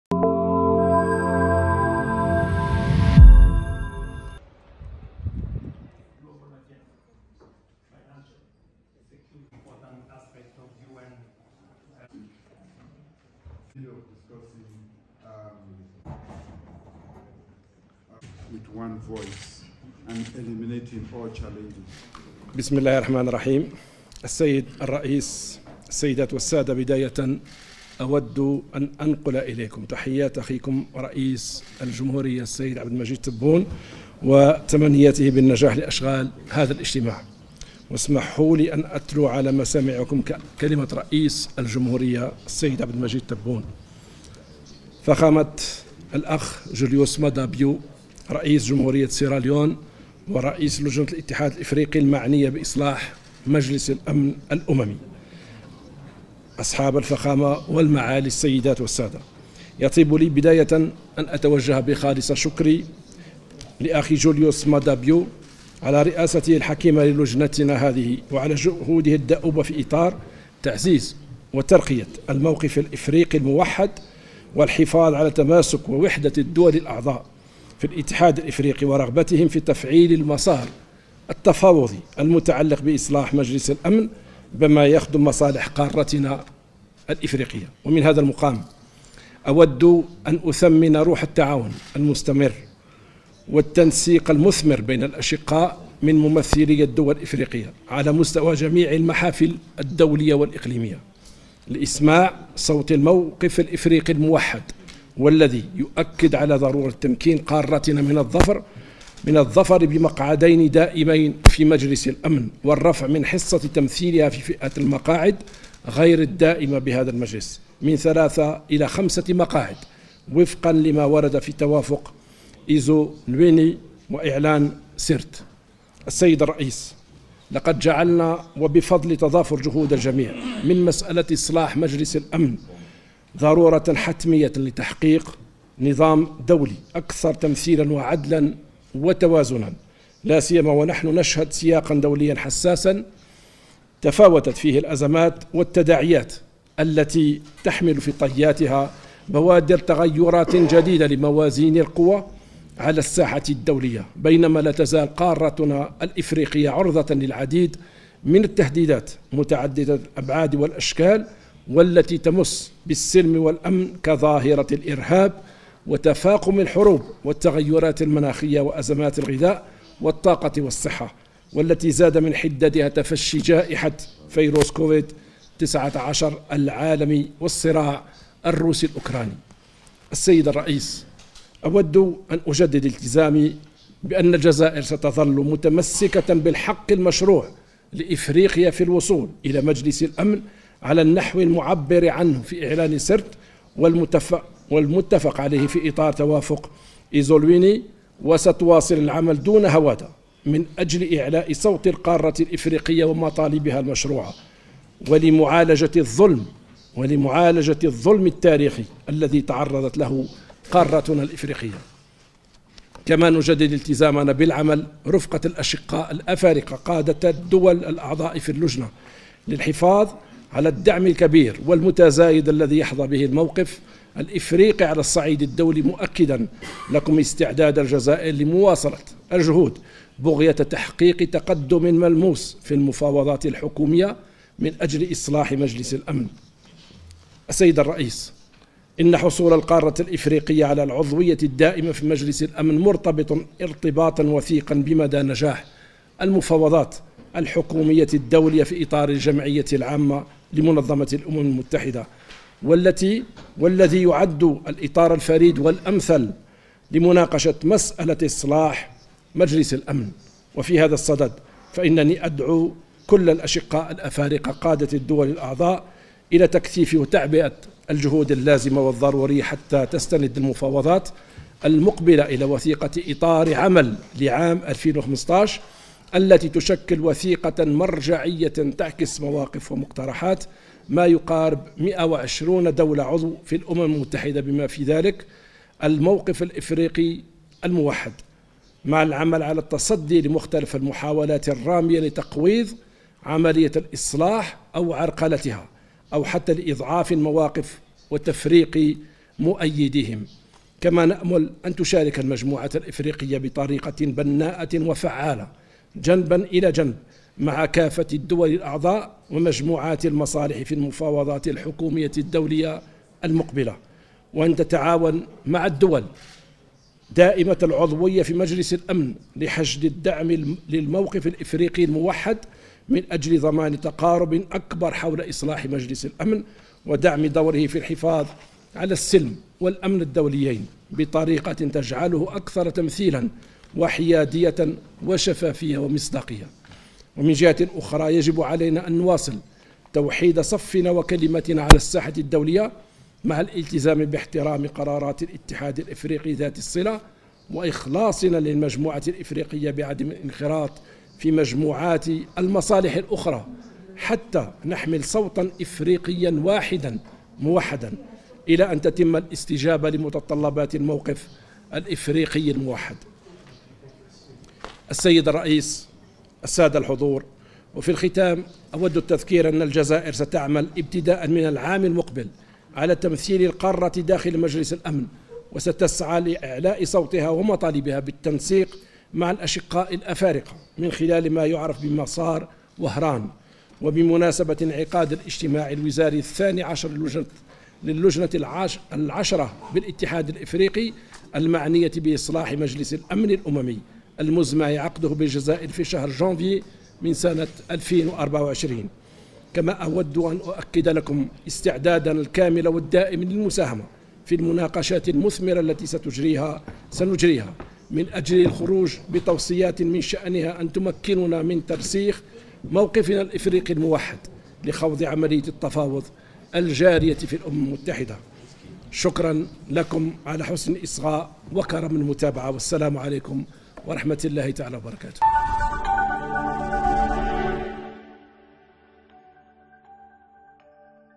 رئيس الجمهورية يجدد تمسك الجزائر بالحق المشروع لإفريقيا في العضوية الدائمة بمجلس الأمن الاممي
أديس أبابا (واج)- جدد رئيس الجمهورية، السيد عبد المجيد تبون، في كلمة تلاها الوزير الأول، السيد ايمن بن عبد الرحمان، خلال اجتماع لجنة العشرة للاتحاد الإفريقي المعنية بإصلاح مجلس الأمن للأمم المتحدة، التزام الجزائر بالتمسك بالحق المشروع لإفريقيا في الوصول إلى العضوية الدائمة بمجلس الأمن الاممي.